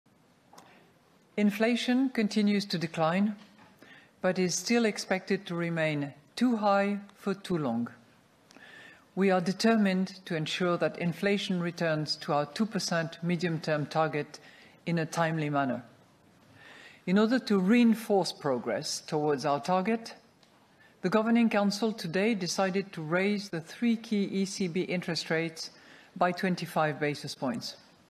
La Bce alza i tassi dello 0,25%. Lagarde: "Inflazione ancora elevata"
L'inflazione continua a diminuire, ma ci si attende tuttora che rimanga troppo elevata per un periodo di tempo troppo prolungato. Il Consiglio direttivo è determinato ad assicurare il ritorno tempestivo dell'inflazione all'obiettivo del 2% nel medio termine. Ha per questo deciso oggi di innalzare di 25 punti base i tre tassi di interesse di riferimento della Bce" le parole della presidente Bce Christine Lagarde in conferenza stampa.